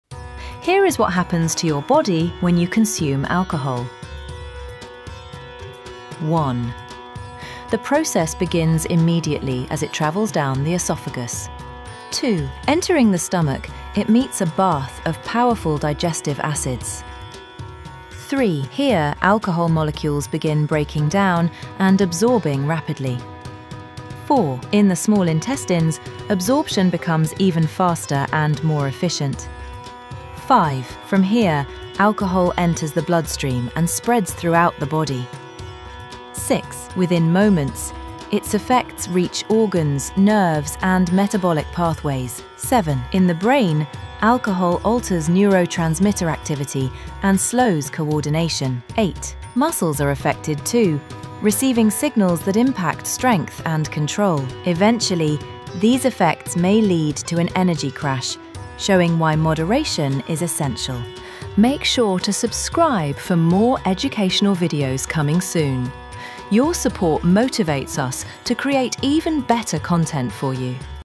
what happens when you drink alcohol everyday
Discover what really happens inside your body when you consume alcohol.
This video visually explains how alcohol moves through your system — from digestion, absorption, and brain effects to the possible risks of overconsumption.

Our goal is to provide clear, science-based education to help you understand how your body reacts and why moderation is important for long-term health.

Your support motivates us to create better content for you!